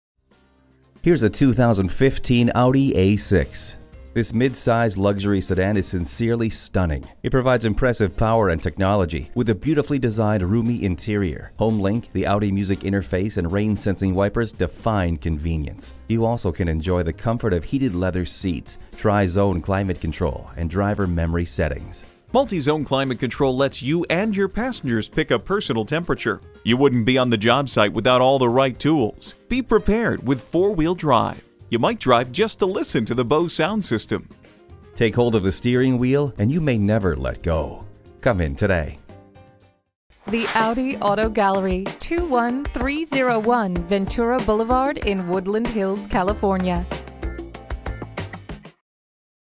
2015 Audi A6 Los Angeles Woodland Hills, CA NAF038313 SOLD
Year: 2015
Make: Audi
Model: A6
Trim: 4dr Sdn quattro 2.0T Premium Plus
Engine: 2.0L TFSI I4 DOHC -inc: Direct-injection and Audi
Transmission: AUTO
Color: BRILLIANT BLACK
Interior: BLACK
Stock #: NAF038313
VIN: WAUGFAFC4FN038313

Address:
21301 Ventura Blvd.